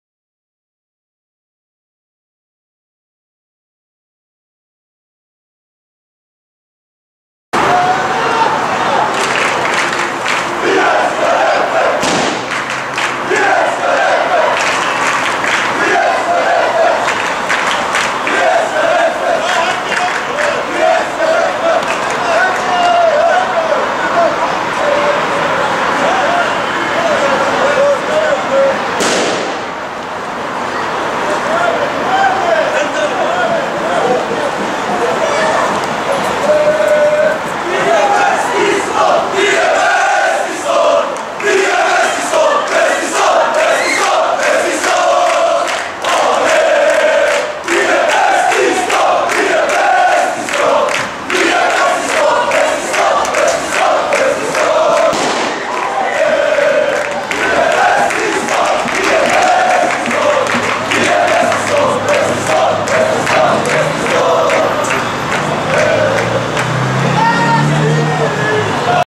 Fansen kommer.wmv
[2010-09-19] AFF-fansen kommer till arenan inför derbyt mellan Assyriska FF - Syrianska FC...